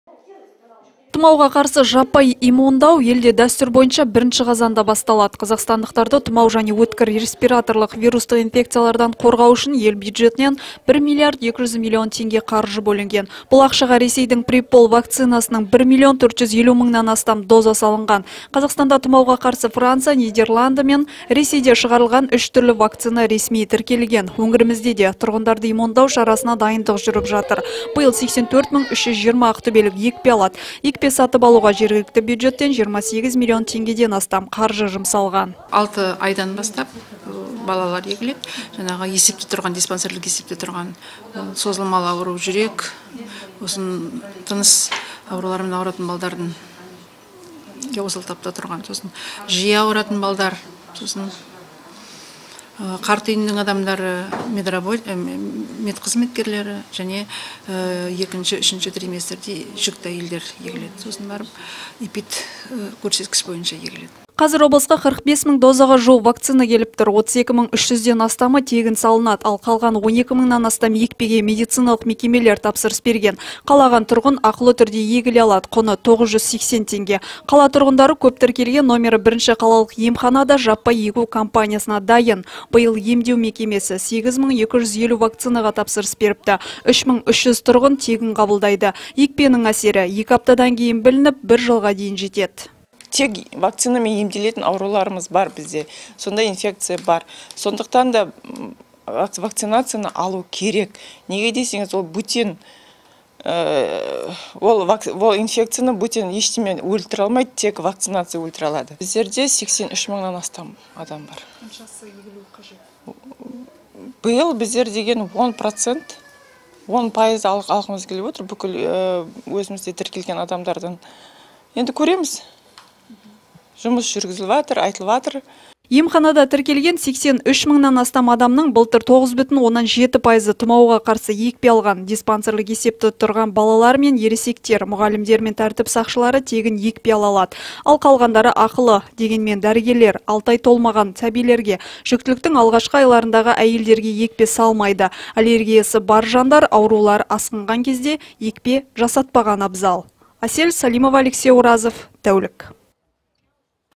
Ақтөбеге тұмауға қарсы вакцина жетті
Қазақстандықтар 1-қазаннан бастап тұмау және өткір респираторлық вирустық инфекцияларға қарсы жаппай егіле бастайды. Ол үшін бюджеттен 1 млрд. 200 млн. теңге бөлінген. Бұл ақшаға  Ресейдің "Гриппол" вакцинасының 1 млн. 450 мыңнан астам дозасы алынған. Қазақстанда тұмауға қарсы Франция, Нидерланды мен Ресейде шығарылған үш түрлі вакцина ресми тіркелген. Өңірімізде де тұрғындарды иммундау шарасына дайындық жүріп жатыр. Биыл 84320 ақтөбелік екпе алады. Екпе сатып алуға жергілікті бюджеттен 28 млн. теңгеден астам қаржы жұмсалған. Қазір облысқа 45 мың дозаға  жуық вакцина келіп тұр. 32 300-ден астамы тегін салынады. Ал, қалған 12 мыңнан астам екпеге медициналық мекемелер тапсырыс берген. Қалаған тұрғын ақылы түрде егіле алады. Құны 980 теңге. Қала тұрғындары көп тіркелген №1 қалалық емхана да жаппай егу кампаниясына дайын. Биыл емдеу мекемесі 8250 вакцинаға тапсырыс беріпті. 3300 тұрғын тегін қабылдайды. Екпенің әсері 2 аптадан кейін білініп, 1 жылға жетеді. Аллергиясы бар жандар аурулары асқынған кезде екпе жасатпағаны абзал.